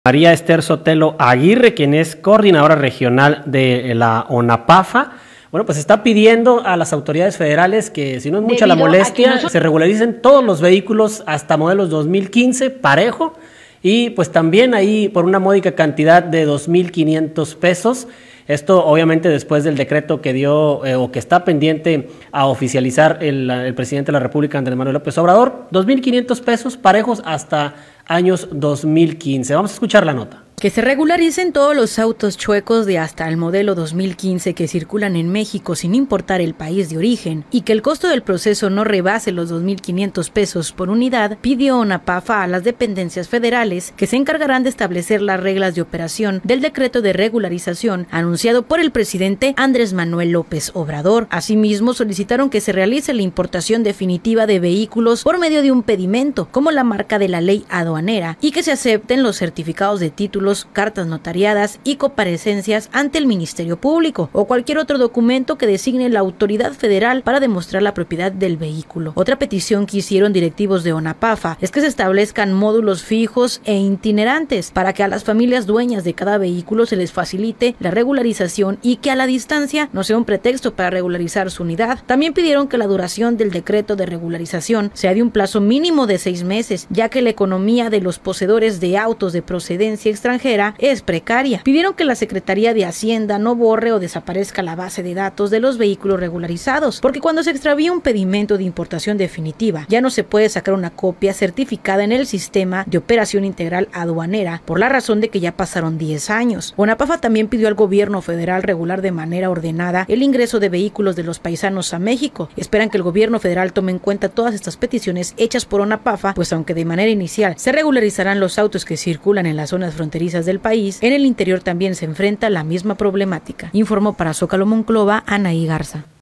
Monclova Que se regularicen todos los autos “chuecos” de hasta modelo 2015 que circulan en México.
Monclova Que se regularicen todos los autos “chuecos” de hasta modelo 2015 que circulan en México sin importar el país de origen, y que el costo del proceso no rebase los 2 mil 500 pesos por unidad, pidió Onappafa a las dependencias federales.